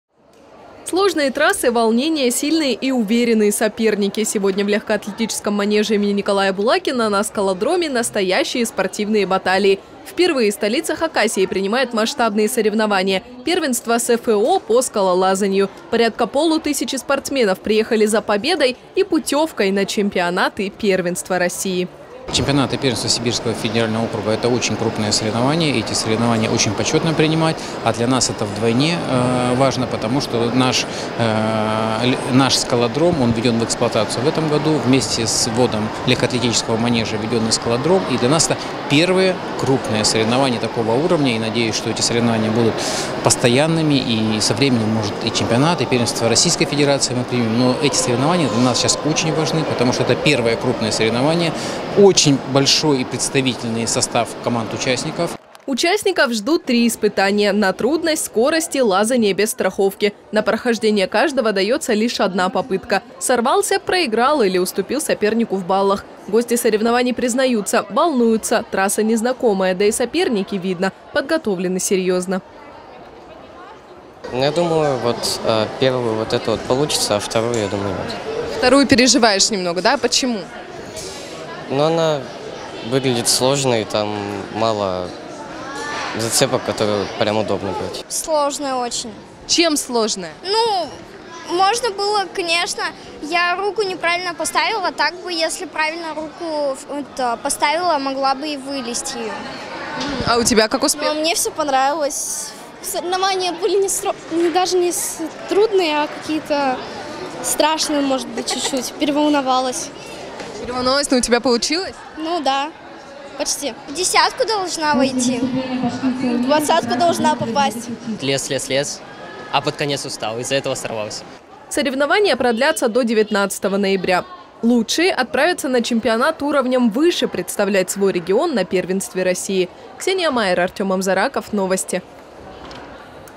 Чемпионат и первенство СФО по скалолазанию стартовал в Абакане - Абакан 24
Ловкость, сила рук, четкий расчет. Сегодня в Абакане впервые был дан старт чемпионату и первенству Сибирского Федерального Округа по скалолазанию. На протяжении пяти дней соревноваться будут более 450 спортсменов. Юные и взрослые альпинисты пройдут 3 испытания: на трудность, скорость и боулдеринг.